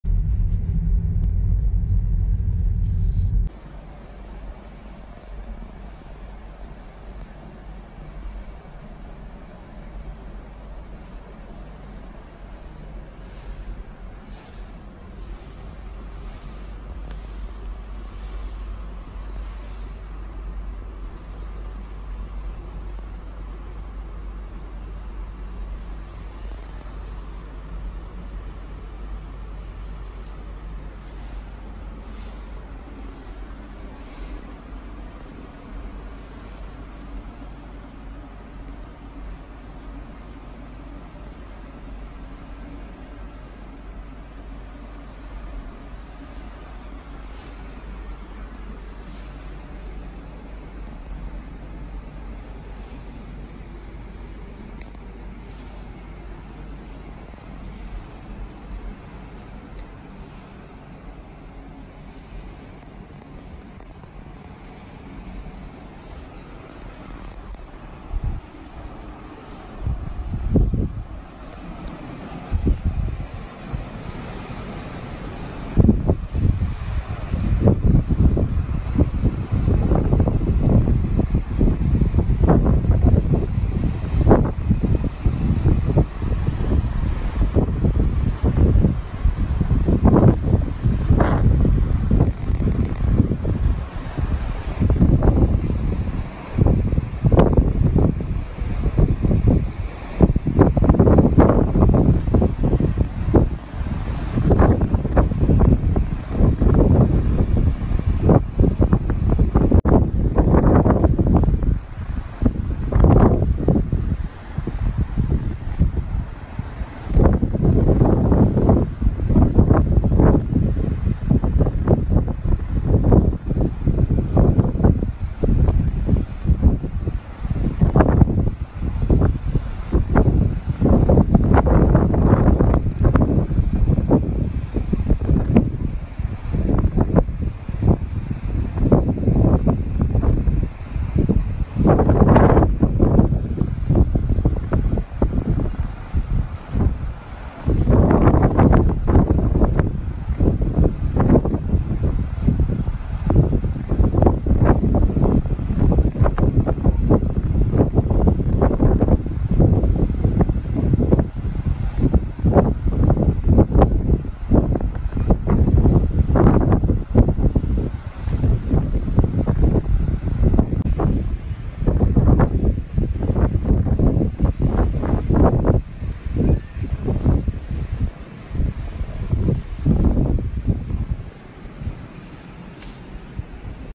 Checking in Hotel de Blanke Top Sky Room Seaview Suites โรงแรม ประเทศเนเธอร์แลนด์
Checking in to Sky Room Seaview Suites at Hotel de Blanke Top, Cadzand-Bad, Zeeland, The Netherlands.โรงแรม ประเทศเนเธอร์แลนด์
🇳🇱พักผ่อนที่ประเทศเนเธอร์แลนด์ เพลย์ลิสต์ ➡️▶️